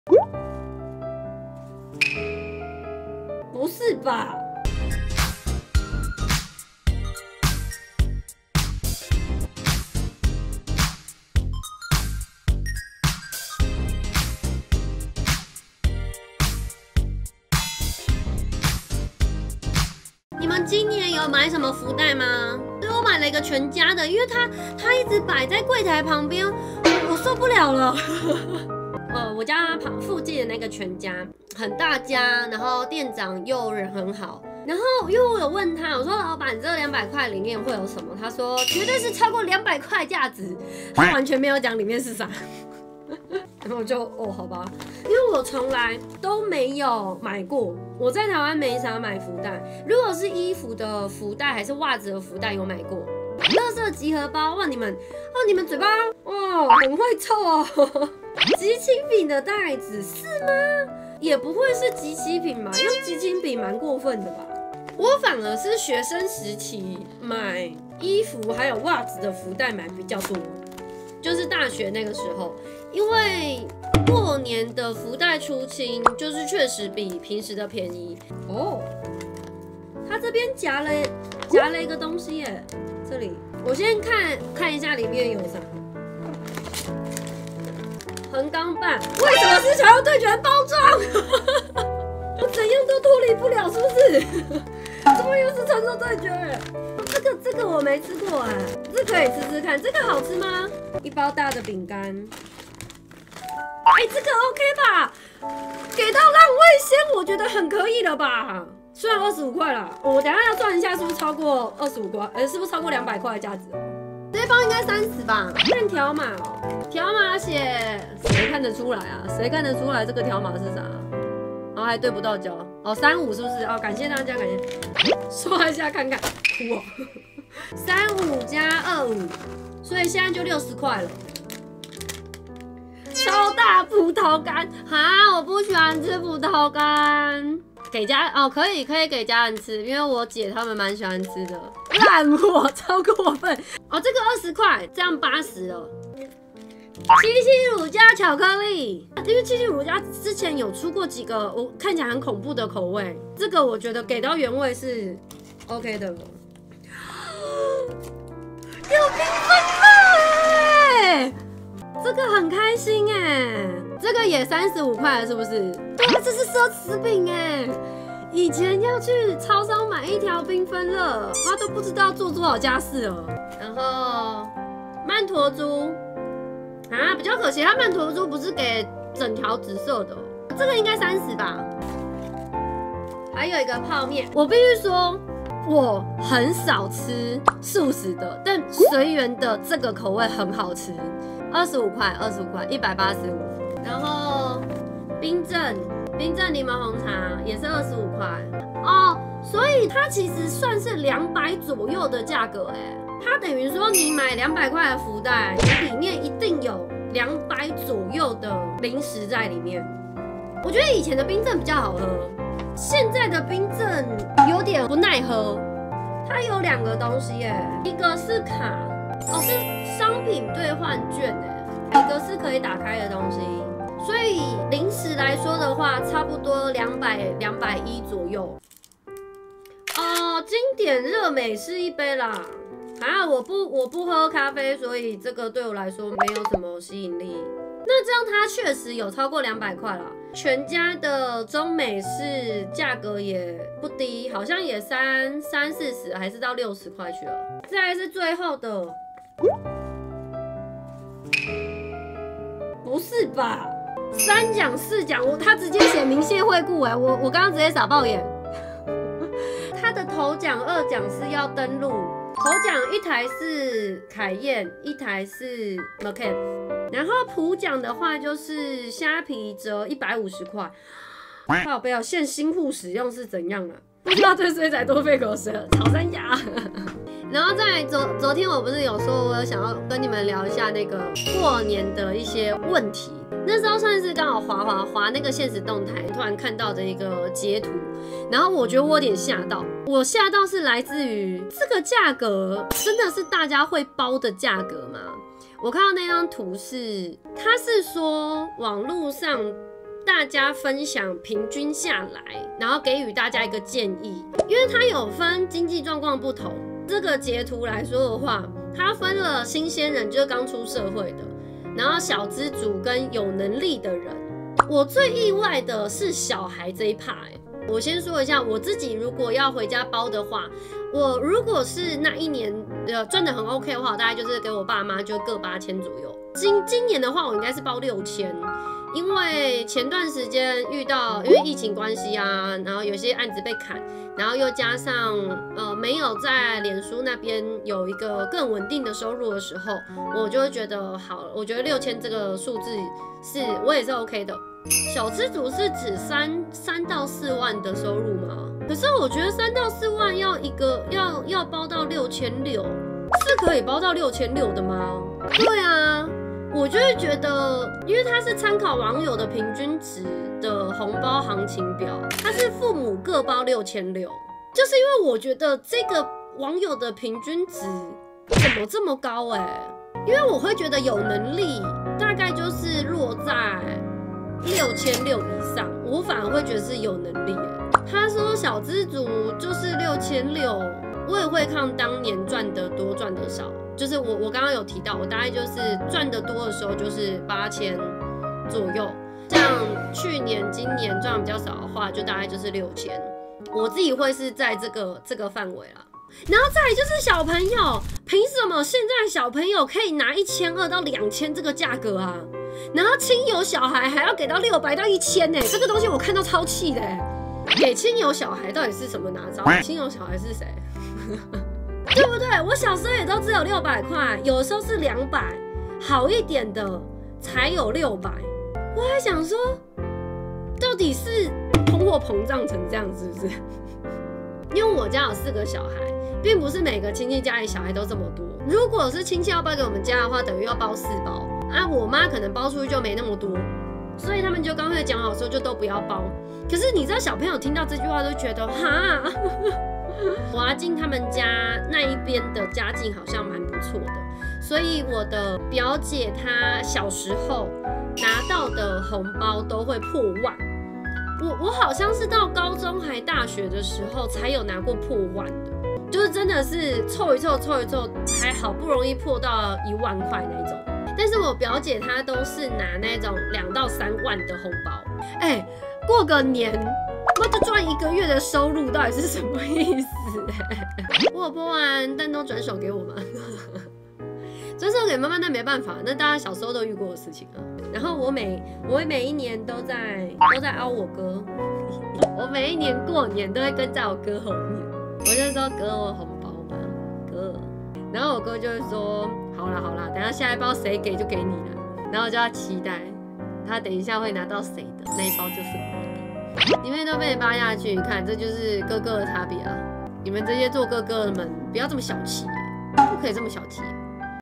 【解憂電台】無業配全家禮包小開箱之最好是現在小孩紅包拿這麼多啦！！怒！
紅包價格看看即可，希望大家量力而為:)
然後誰可以跟我說說 親友小孩憑什麼給我拿這麼多紅包呢?

★實況都搬到圖奇嚕★
搜尋：raby7721 瑞比列車長